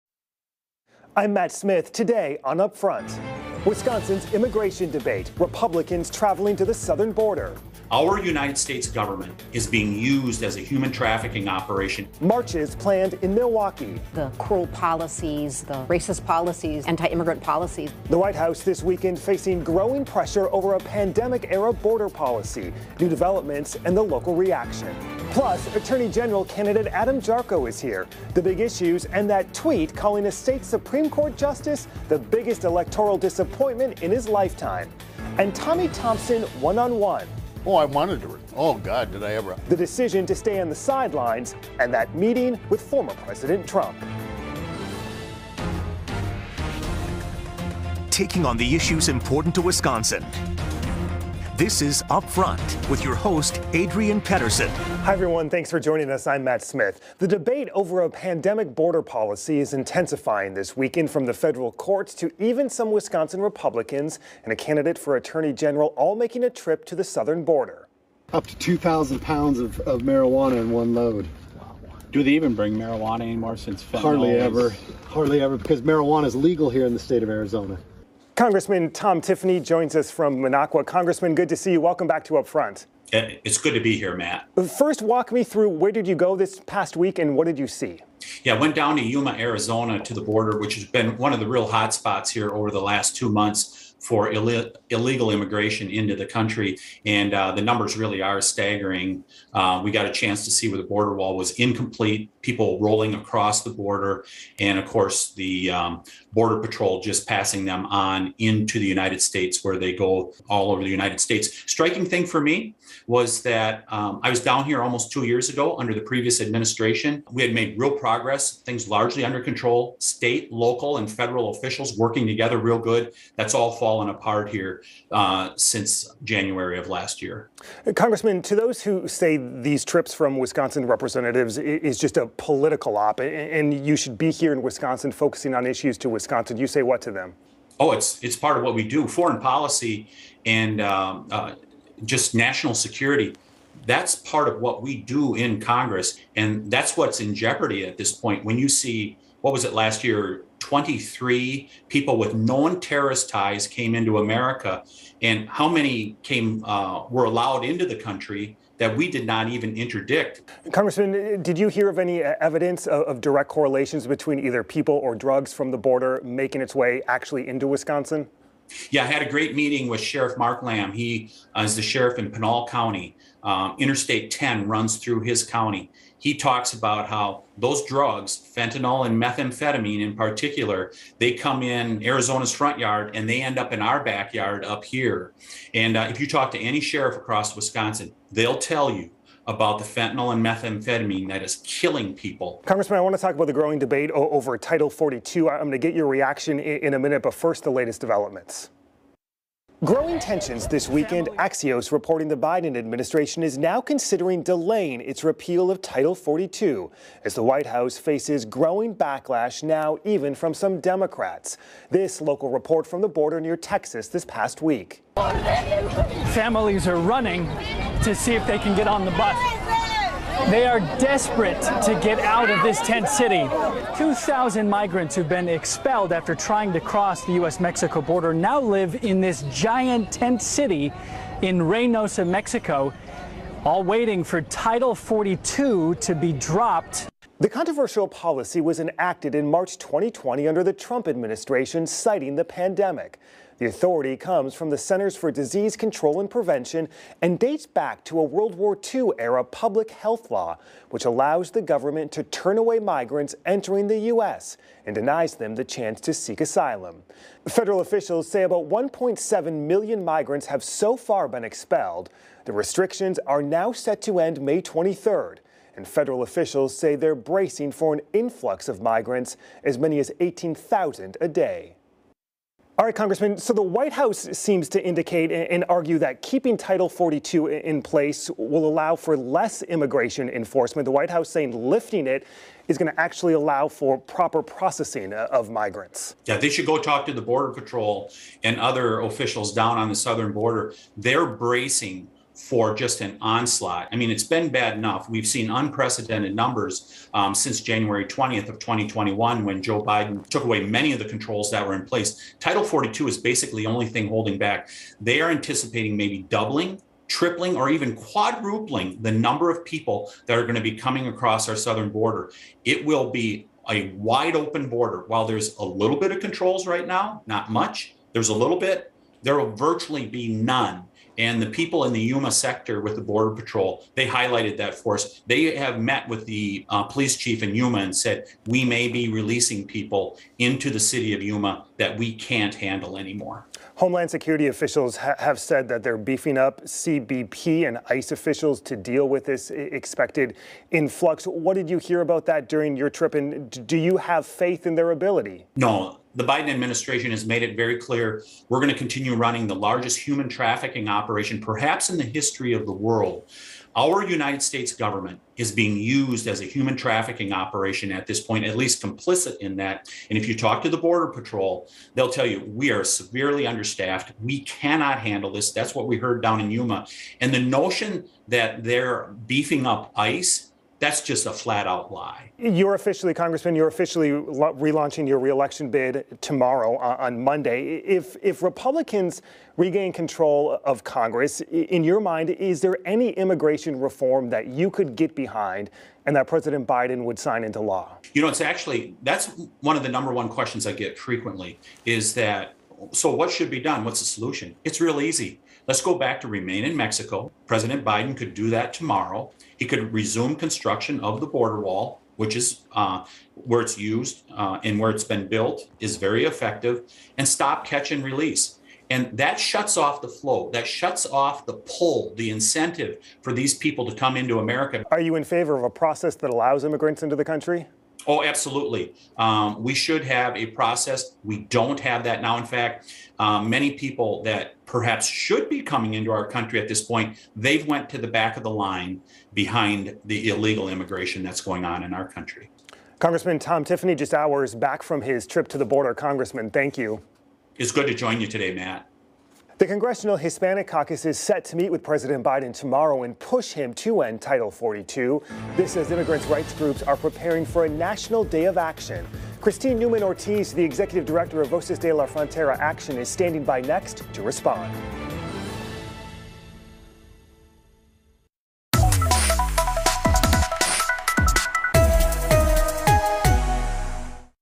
Rep. Tiffany visits southern border
Republican warns of repealing Title 42